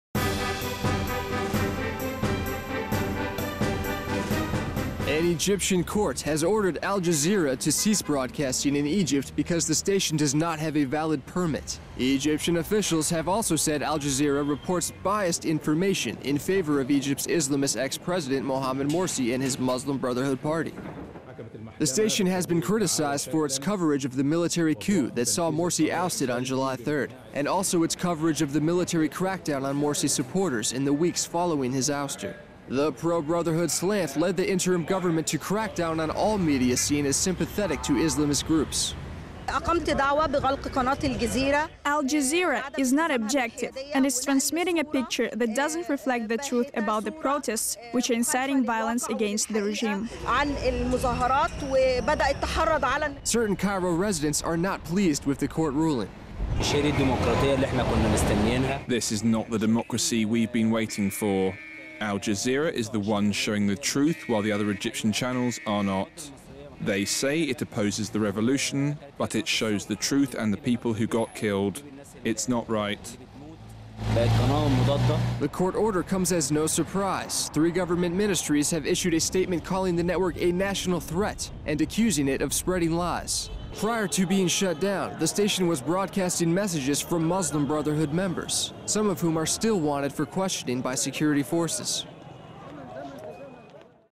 Egypt bans Al Jazeera: TV news channel forced off air amid claims of pro-Muslim Brotherhood bias
An Egyptian court has ordered Al Jazeera to cease broadcasting in Egypt because the station does not have a valid permit. Egyptian officials have also said Al-Jazeera reports biased information in favour of Egypt's Islamist ex-President Mohammed Morsi and his Muslim Brotherhood party.